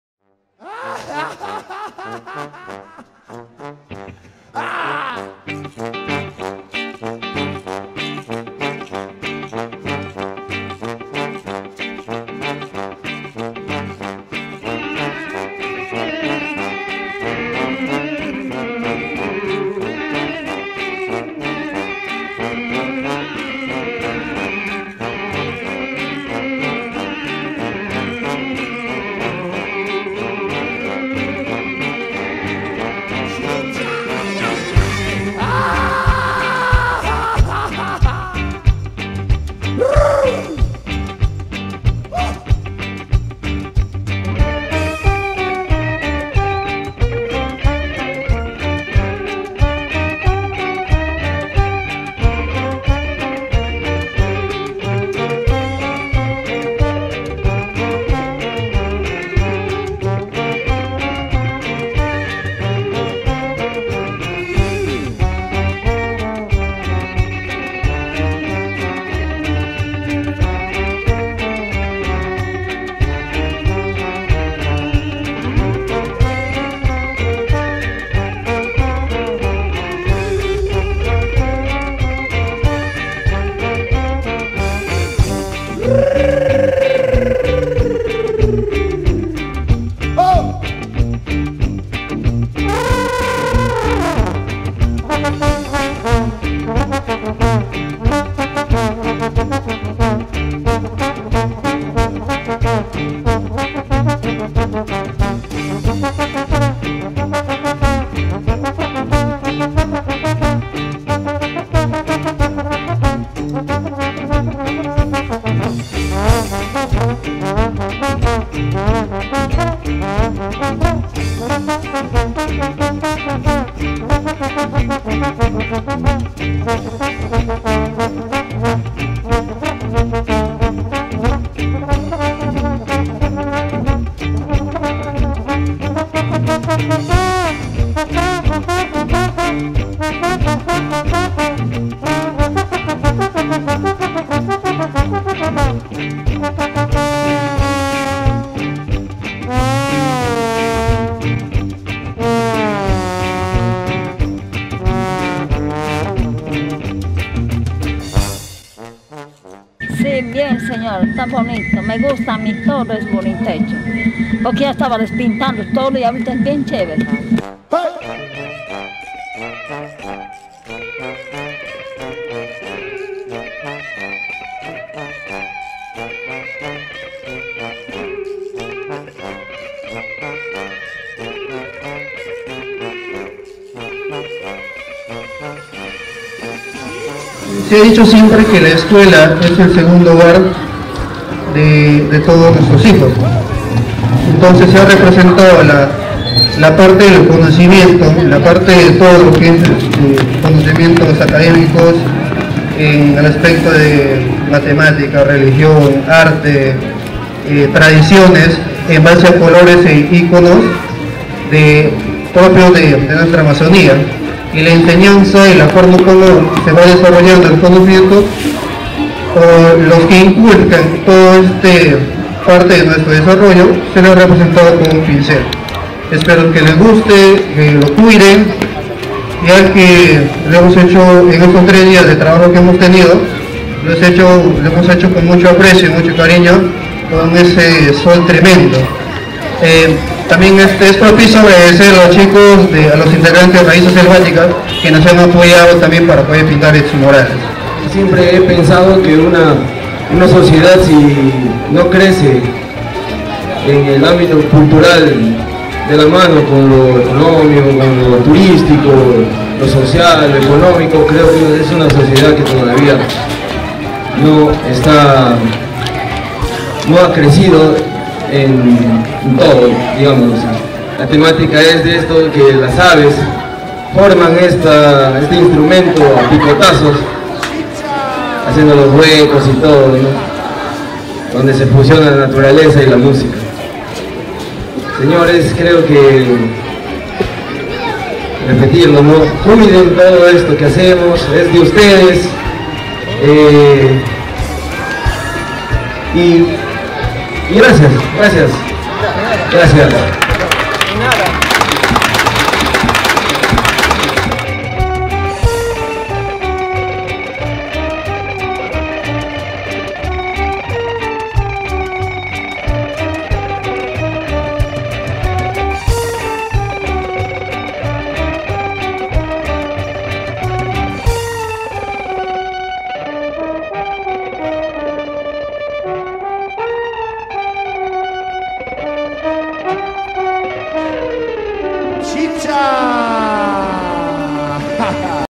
Muralización Nacional -Tarapoto
Muralización Nacional es un colectivo de artistas dedicados a la pintura urbana en la ciudad de Tarapoto - Perú.
Generalmente sus intervenciones son en colegios, murales y en los lugares mas comunes  en donde la propaganda política pretenda contaminar los espacios.

Vídeo realizado por Aurea Producciones Perú
                                Mayo - 20014